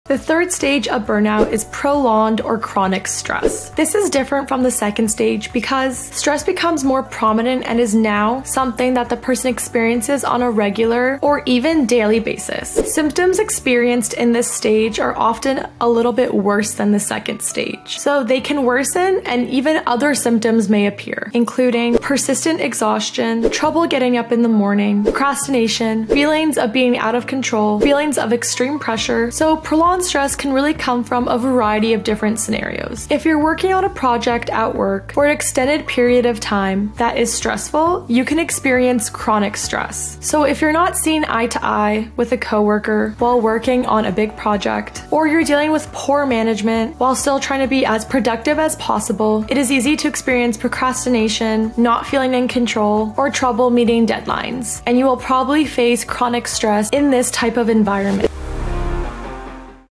When BURNOUT turns into CHRONIC Stress
In 2019, the world health organization classified workplace burnout as an occupational phenomenon. Unlike fatigue, burnout affects your emotional health and reduces your self-efficacy.

Since then, the emphasis on work burnout and spreading awareness of this issue, has skyrocketed, and for good reason. Your body knows when you’re burned out, so are you going to listen?

🧰 Tools & Resources:

What is Uvaro? Start here!